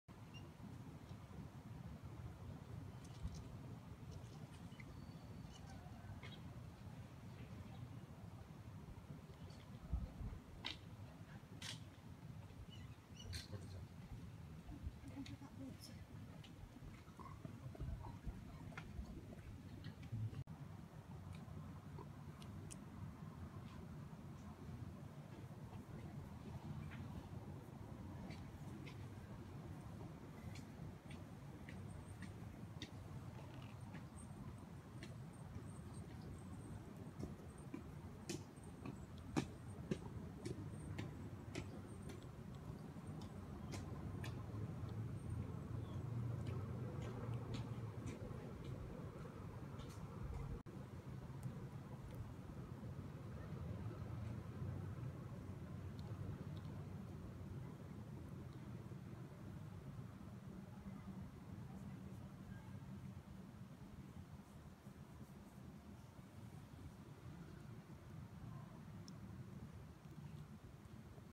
猫708 なでなで off time
なでなでさせてくれた黒ヌコ

癒されます